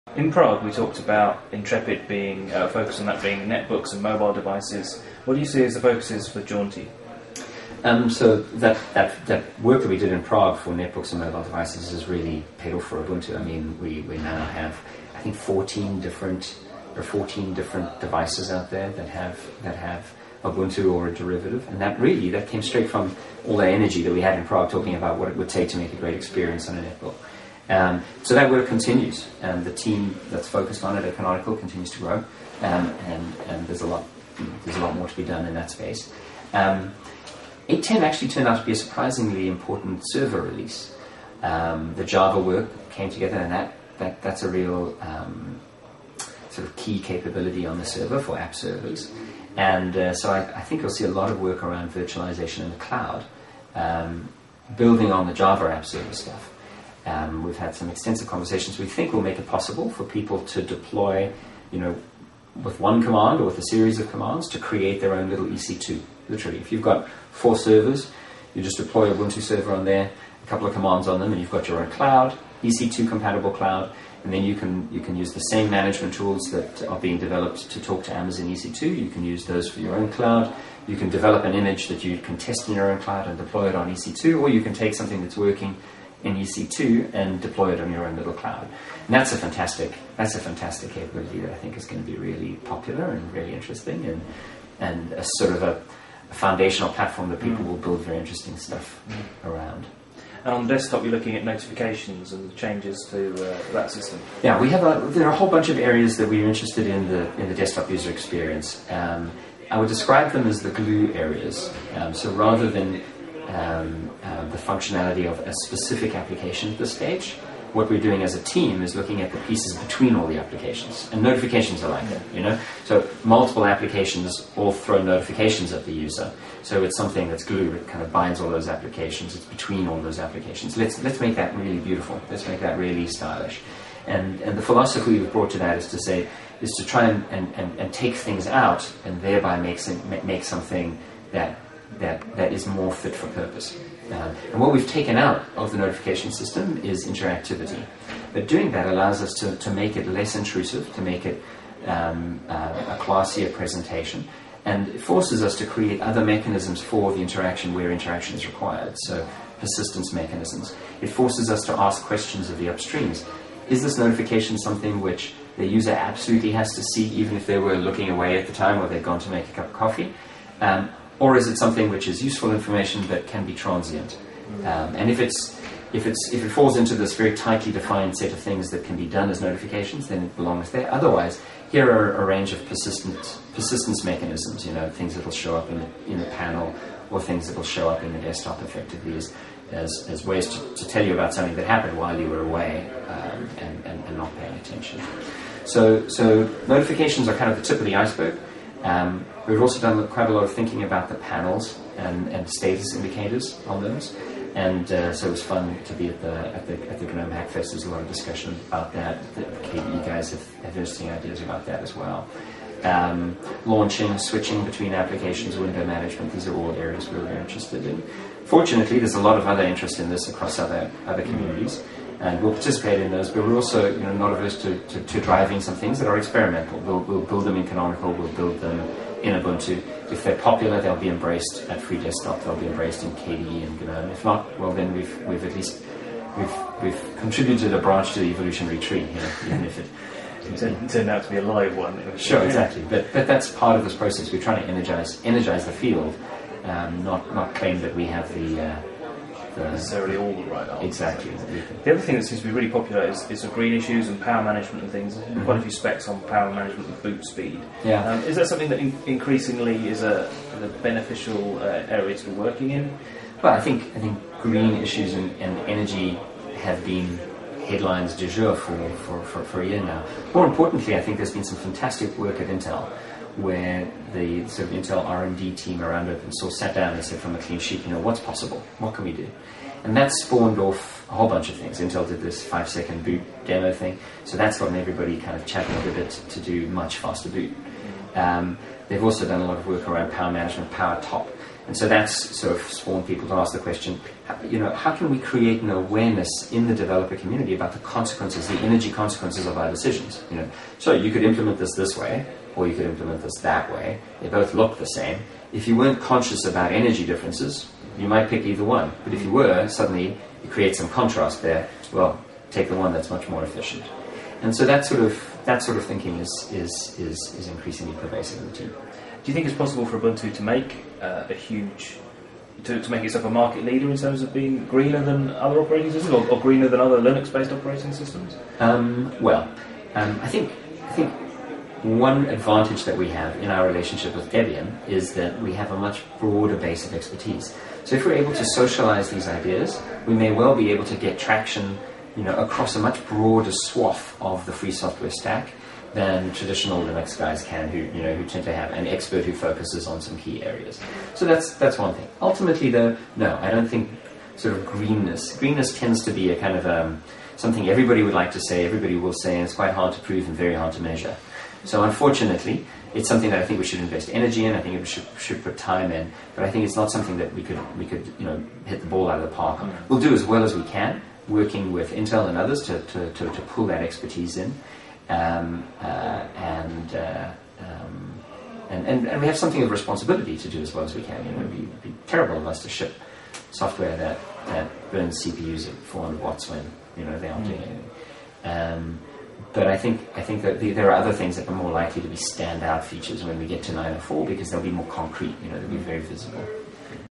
UDS Jaunty - Mark Shuttleworth (Part Two)
Mark Shuttleworth talks about UDS, how the new teams at Canonical will work, the impact of changes to the way Ubuntu is developed and the development focus for Jaunty.